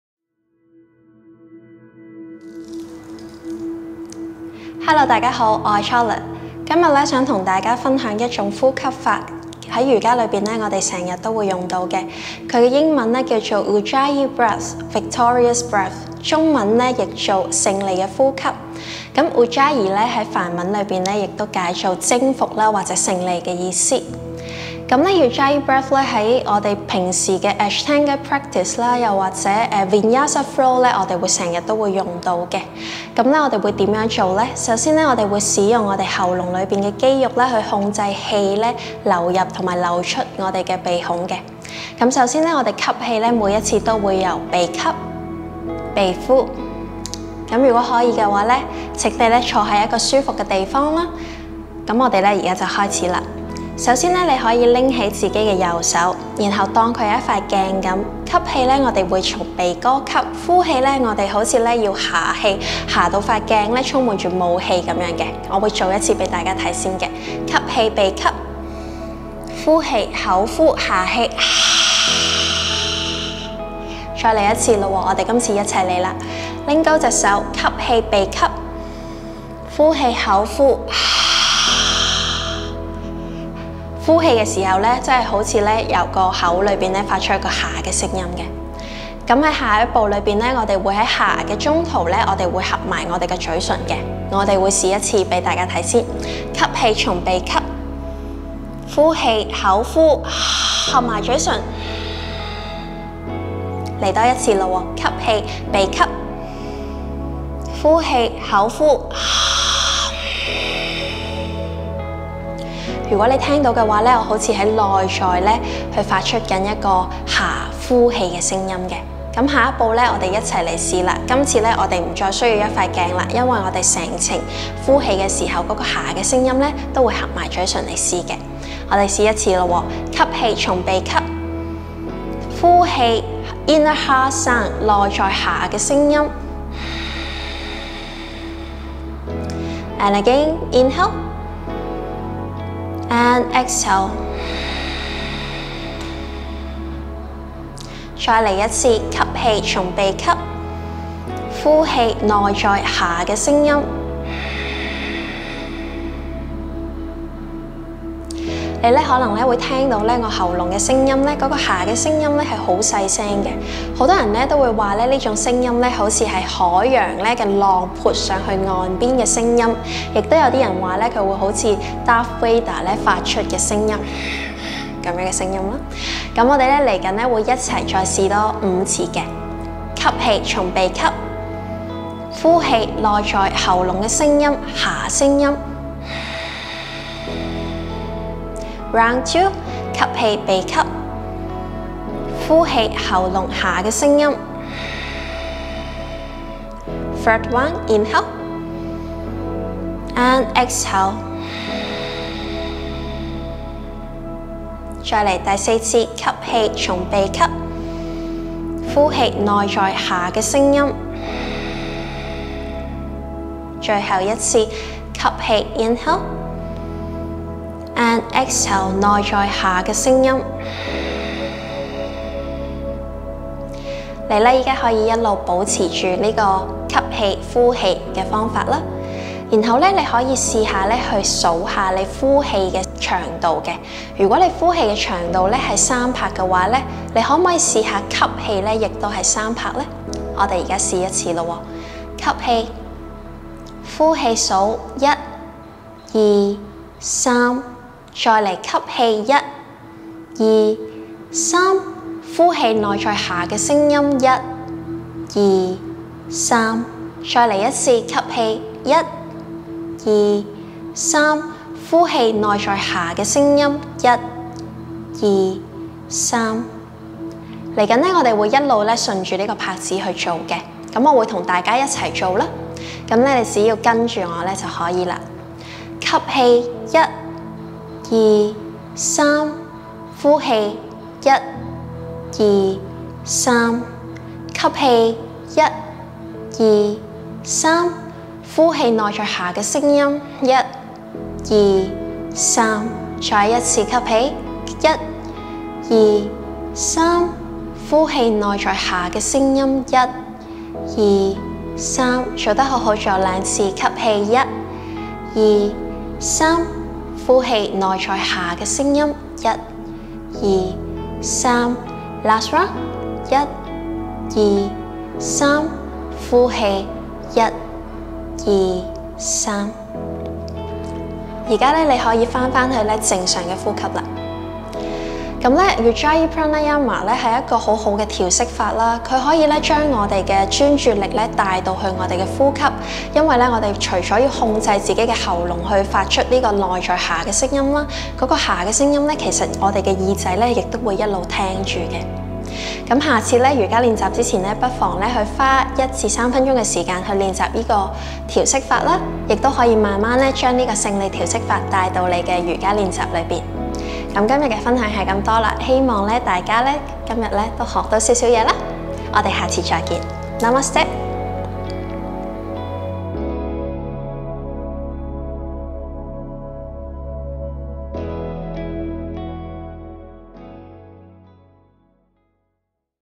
勝利呼吸法教學 | Ujjayi Breath Pranayama Tutorial
Meditation | 冥想

【Ujjayi Breath Pranayama】
Ujjayi Pranayama, victorious breath, is the breathing exercise which is common in yoga practice, helps you to concentrate in yourself.

【勝利呼吸法】
常用的瑜伽呼吸法。通過控制你的呼吸與意念，並隨時提醒自己專注於當下。